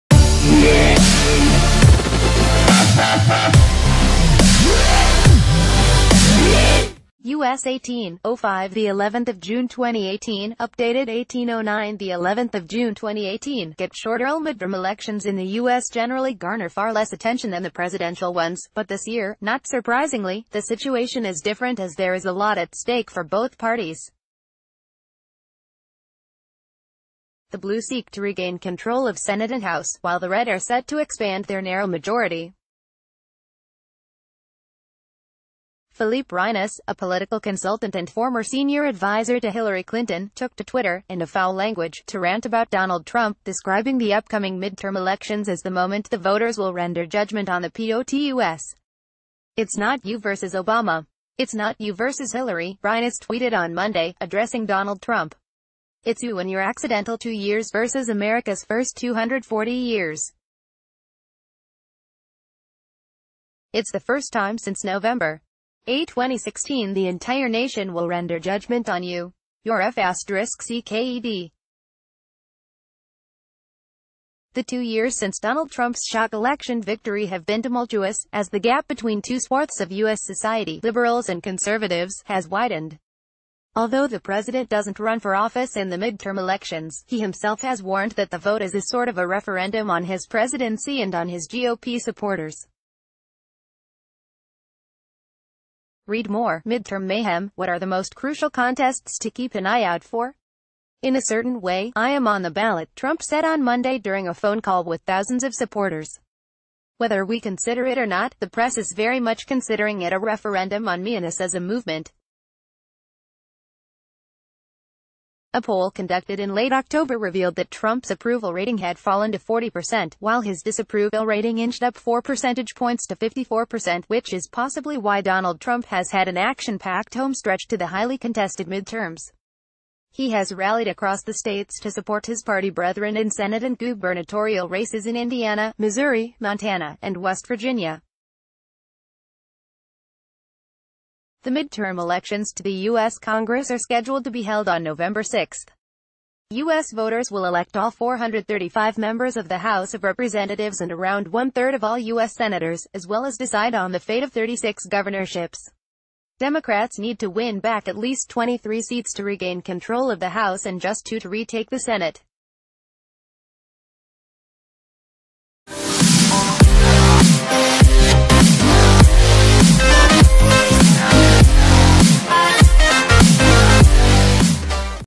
Daily News - Youre F * cked: Former Clinton Prime Minister Aide Berates Trump as Midterms Heat Up
US18:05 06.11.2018(updated 18:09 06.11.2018) Get short URLMidterm elections in the US generally garner far less attention than the presidential ones, but this year, not surprisingly, the situation is different as there is a lot at stake for both parties. The Blue seek to regain control of Senate and House, while the Red are set to expand their narrow majority. Philippe Reines, a political consultant and former senior advisor to Hillary Clinton, took to Twitter — and to foul language — to rant ab...